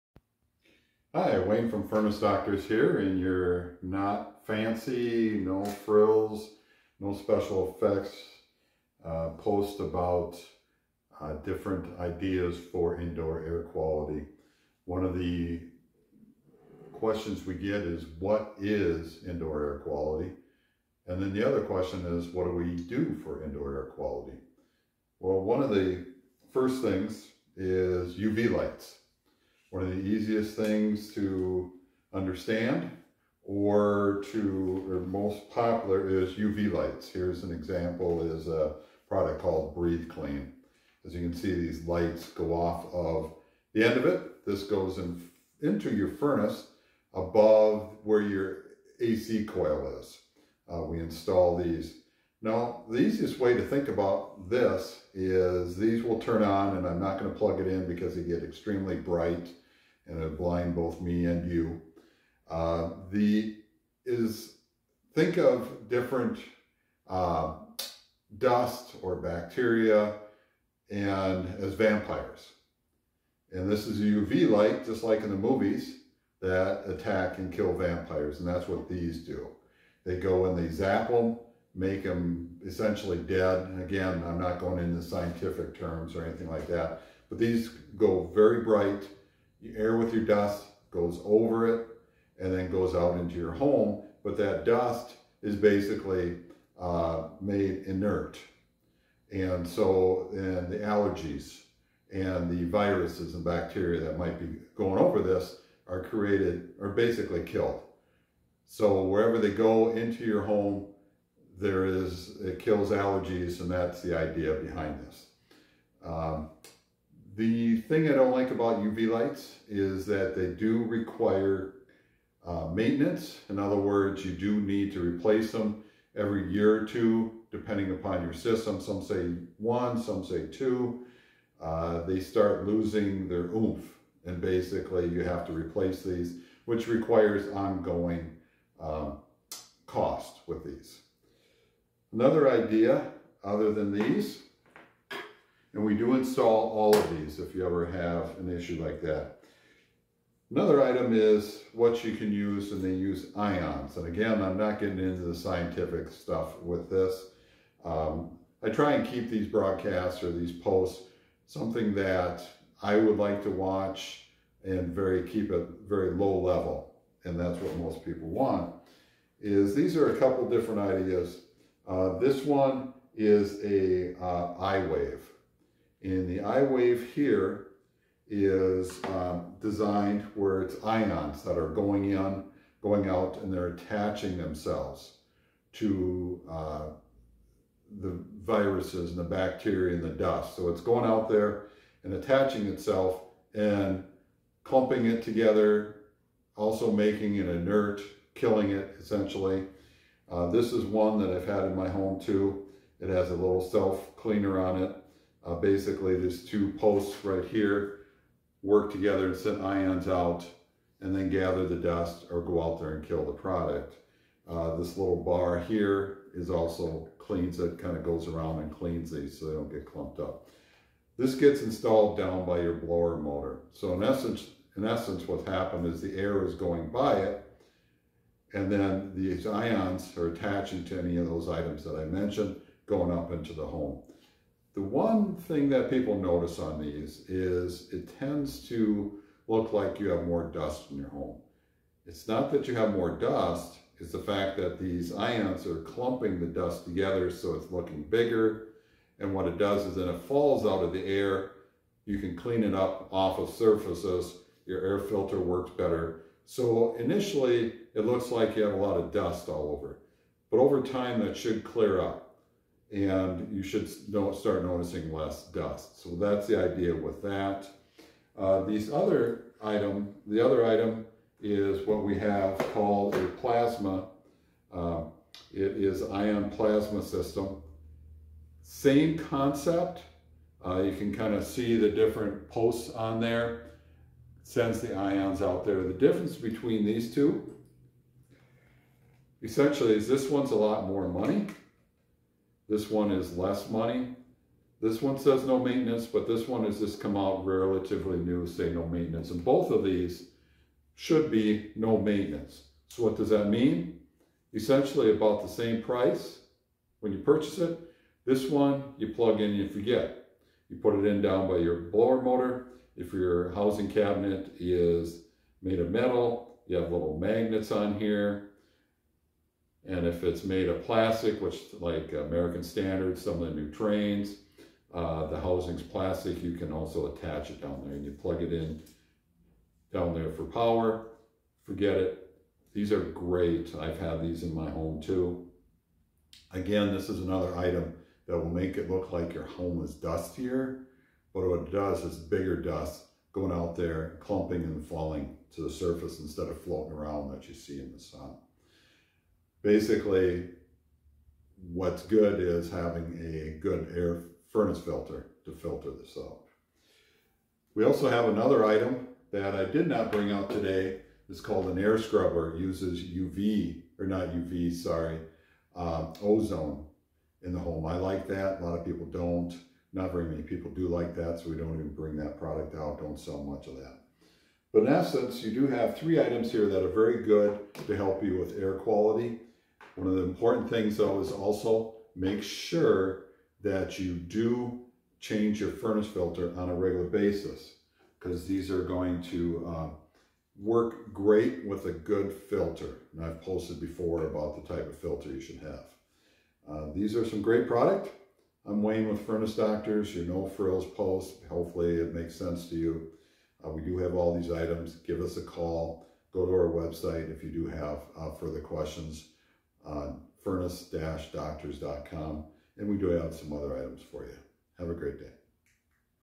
What is the difference between UV Lights and ION Units for you air ducts? I will try to explain.
There are many tools to help with indoorairquality in your home. Popular tools are uvlight and ION based. What is the difference?

My video will show examples of UV lights and ION Plasma methods of helping to eliminate dust, bacteria, mold, mildew, viruses etc.  Different methods had different effects.